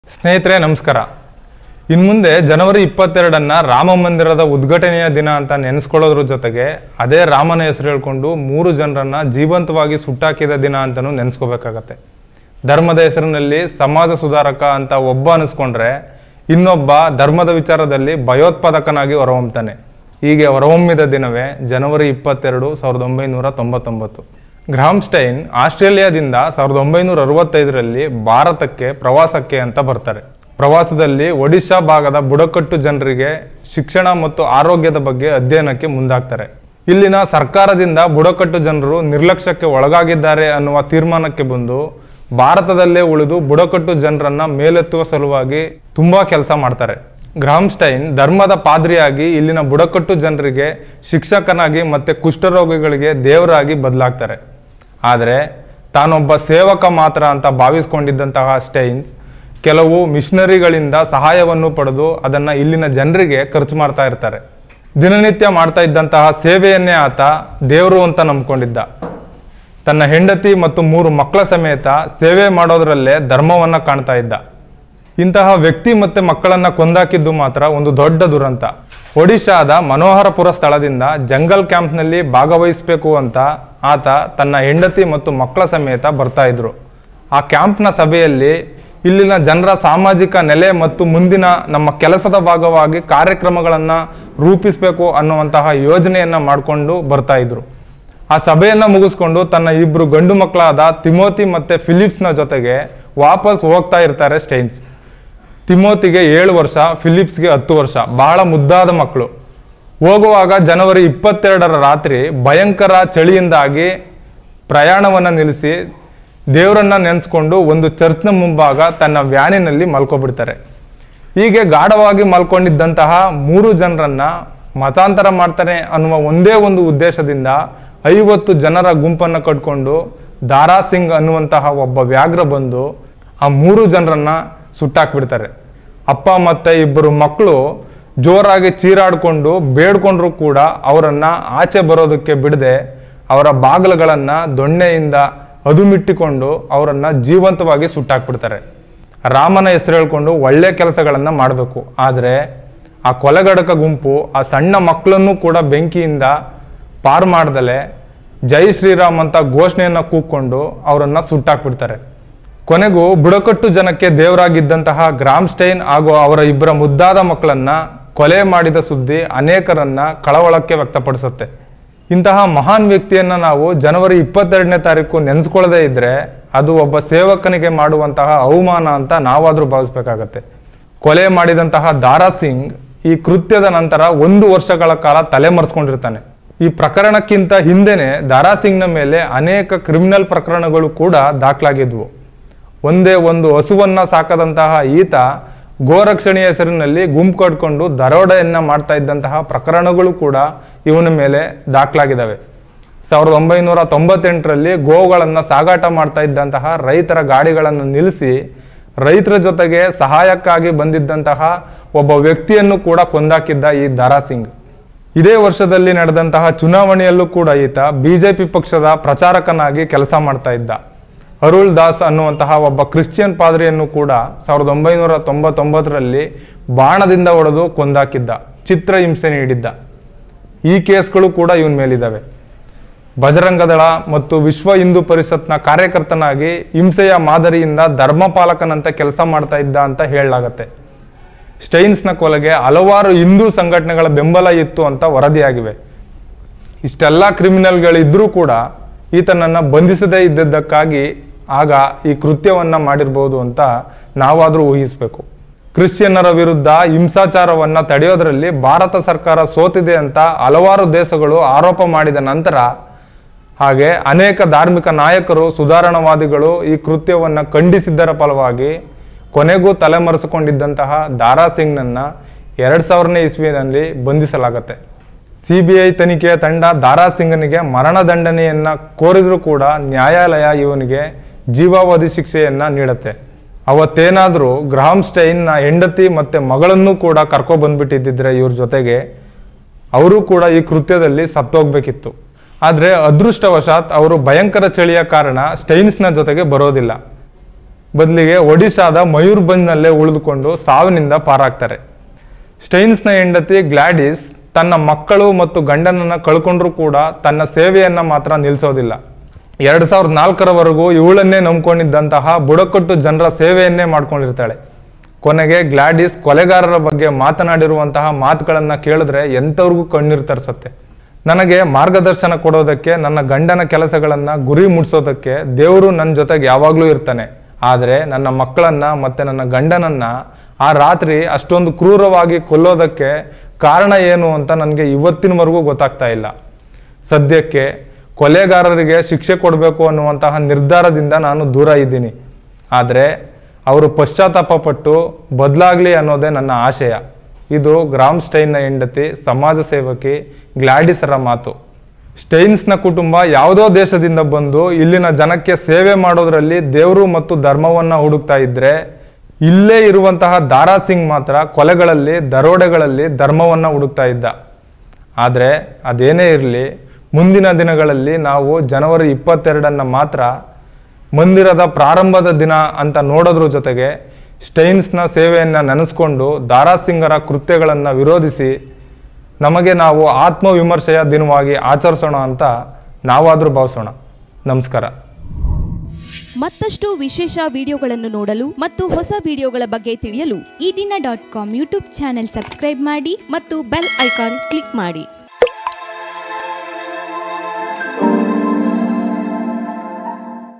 ಜ.22 - ರಾಮ ಪ್ರಾಣ ಪ್ರತಿಷ್ಠ ದಿನ ಮಾತ್ರವಲ್ಲ; ಮೂವರನ್ನು ಕೊಂದ ದಿನ
1965 ರಲ್ಲಿ ಭಾರತದ ಪ್ರವಾಸಿಗನಾಗಿ ಬಂದಂತಹ ಗ್ರಹಾಂ ಸ್ಟೇನ್ಸ್, ಒಡಿಶಾ ಭಾಗದಲ್ಲಿನ ಆದಿವಾಸಿ ಬುಡಕಟ್ಟುಗಳ ದಾರುಣ ಪರಿಸ್ಥಿತಿಯನ್ನು ಮನಗಂಡು ಇಲ್ಲೇ ನೆಲೆಸಿ, ಬುಡಕಟ್ಟು ಜನರ ಶಿಕ್ಷಣ ಮತ್ತು ಆರೋಗ್ಯ ಸುಧಾರಣೆಯಲ್ಲಿ ಮಹತ್ವದ ಕೆಲಸ ಮಾಡಿದರು. ಕೊನೆಗೆ ಕ್ರಿಶ್ಚಿಯನ್‌ ಪಾದ್ರಿಯಾಗಿ ಹಲವಾರು ಕುಷ್ಠರೋಗಿಗಳನ್ನು ಪಾಲನೆ ಮಾಡಿದ್ದಾರೆ. ಆದರೆ ಜನವರಿ 22 ರಂದು ಭಜರಂಗದಳ ಹಾಗು ವಿಶ್ವ ಹಿಂದೂ ಪರಿಷತ್‌ ಕಾಲಾಳಾದ, ದಾರಾ ಸಿಂಗ್ 50 ಗುಂಪನ್ನು ಕಟ್ಟಿಕೊಂಡು ಬಂದು ಗ್ರಹಾಂ ಸ್ಟೇನ್ಸ್ ಮತ್ತು ಇಬ್ಬರು ಮುದ್ದಾದ ಮಕ್ಕಳನ್ನು ಬೆಂಕಿ ಹಾಕಿ ಕೊಂದುಬಿಟ್ಟ.